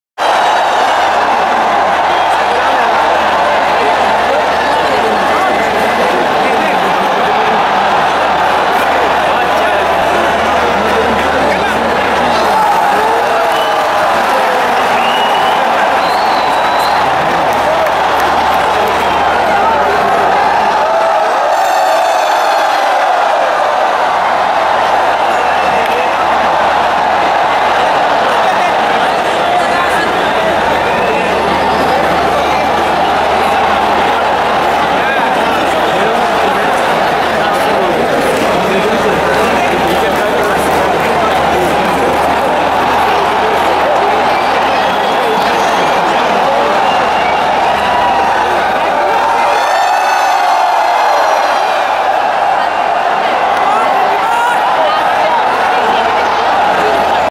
Mexican wave at Eden | First Pink Ball Test | India's First Day Night Test | Eden Garden
@ Eden Garden...1st Pink Ball Day Night Test match in India on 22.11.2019